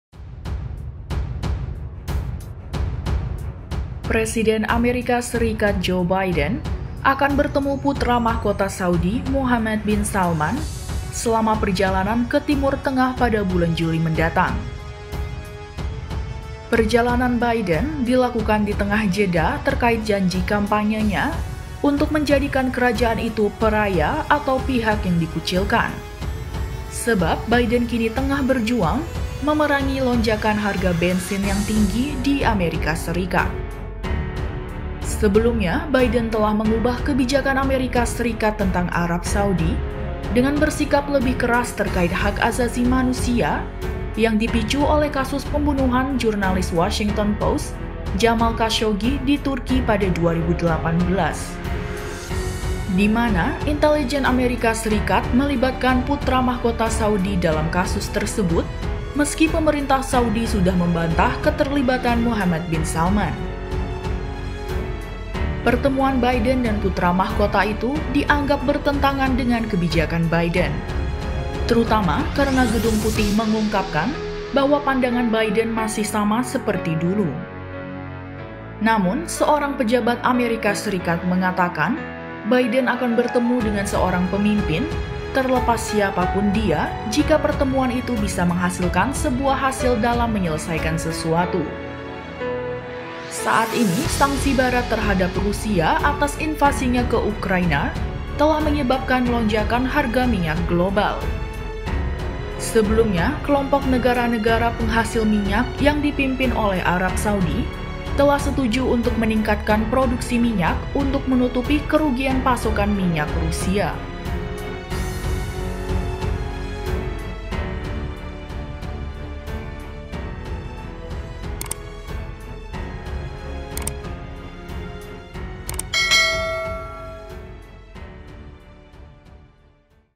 Pertemuan dengan Putra Mahkota Saudi, Biden Dianggap Tak Konsisten
Presiden Amerika Serikat Joe Biden akan bertemu Putra Mahkota Saudi Mohammed bin Salman selama perjalanan ke Timur Tengah pada bulan Juli mendatang.

Perjalanan Biden dilakukan di tengah jeda terkait janji kampanyenya untuk menjadinya kerajaan itu “paria” atau pihak yang dikucilkan, sebab Biden kini berjuang memerangi lonjakan harga bensin yang tinggi di AS.

Penulis Naskah: Novinda Sekar Putri
Narator: Novinda Sekar Putri
Video Editor: Agung Setiawan
Produser: Agung Wisnugroho